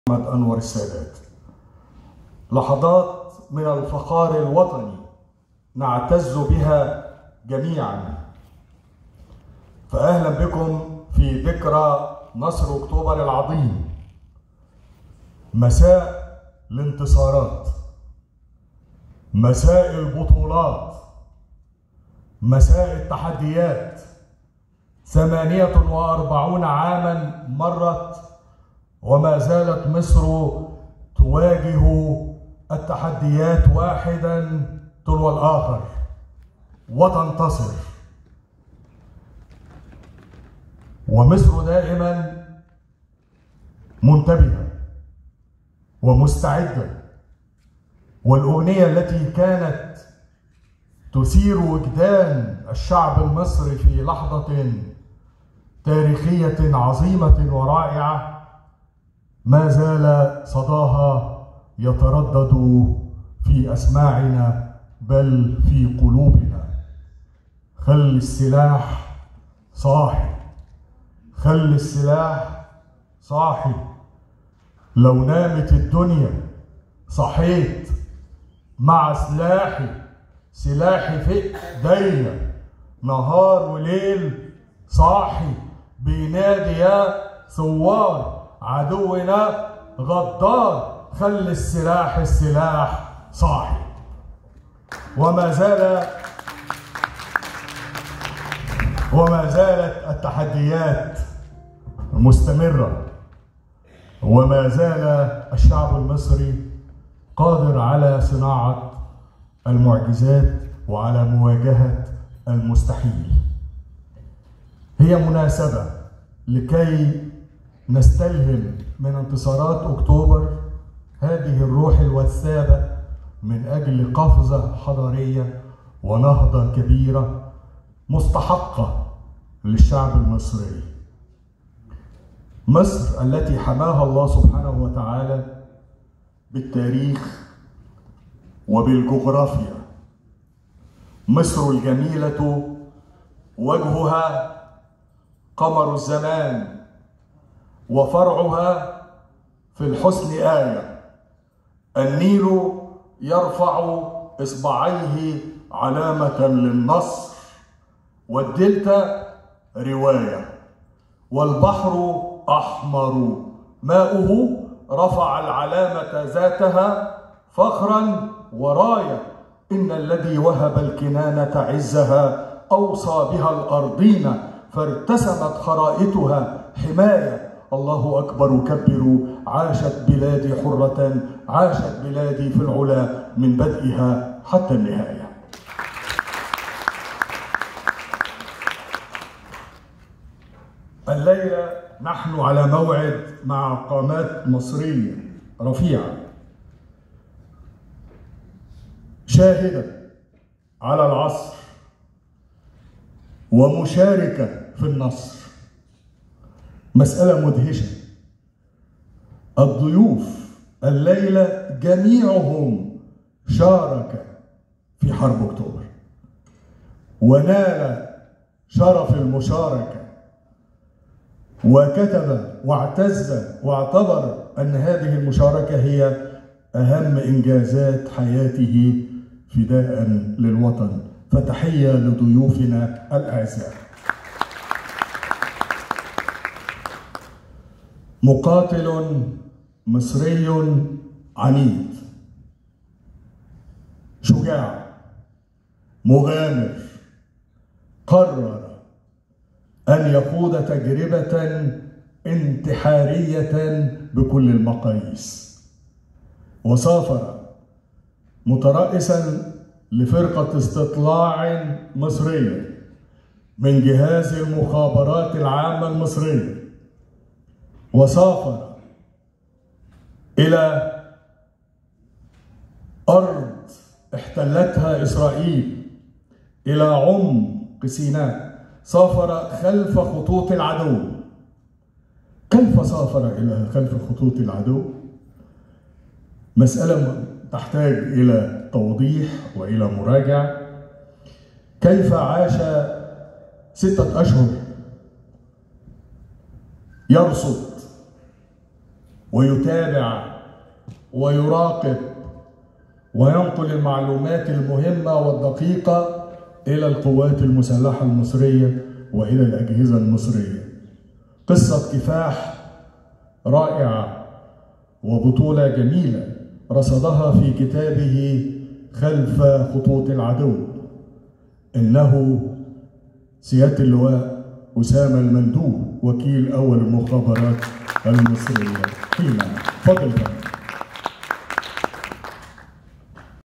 احتفالات نصر اكتوبر تاريخ صنع مستقبل
احتفالات نصر اكتوبر
ندوة تاريخ صنع مستقبل
ضيوف الندوه اللواء اسامة المندوه
ا.د عبد المنعم سعيد
الفنان ا.د احمد نوار
ويدير الحوار جمال الشاعر